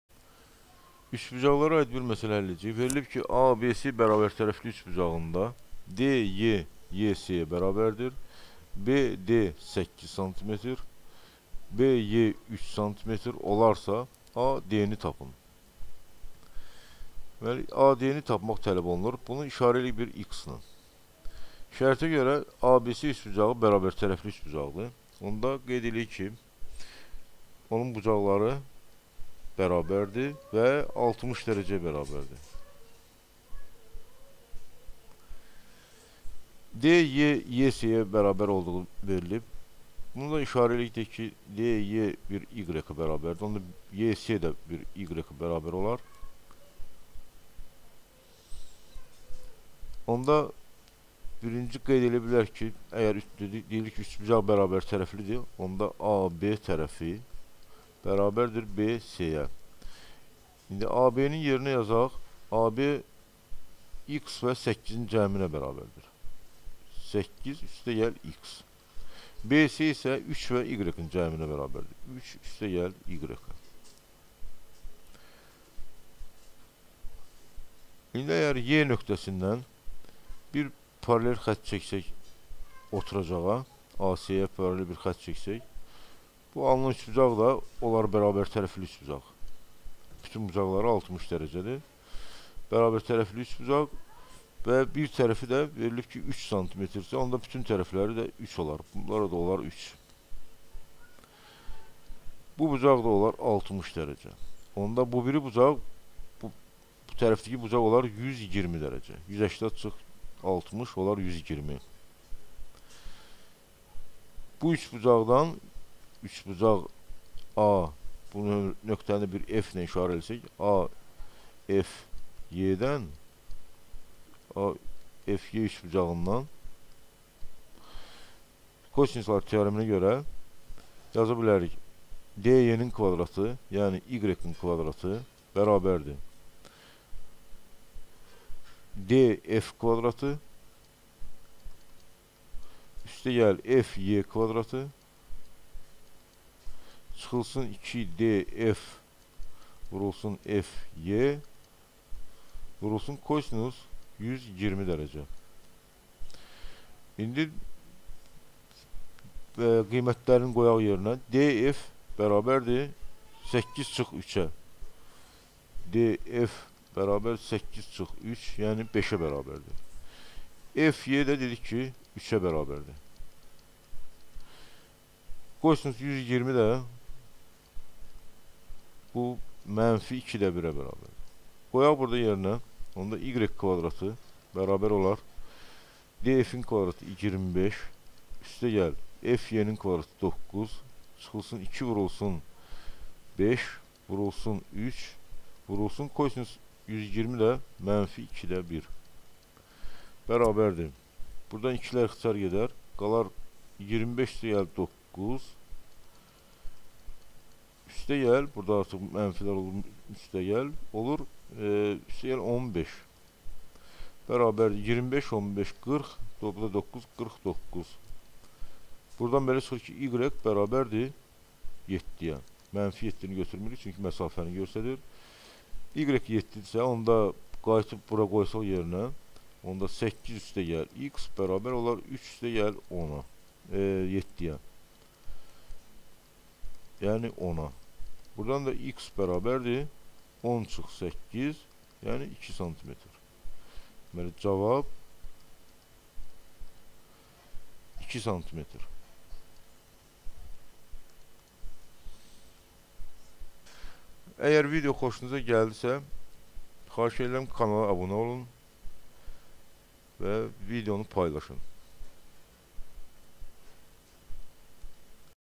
DE=EC ,BD=8sm ,BE=3sm olarsa AD-ni tapın.
Üçbucaqlar.Mənbə:Güvən 2021.üçbucaqlar.Test B
1:57 -de üçbucaq AFE yox DFE- dir.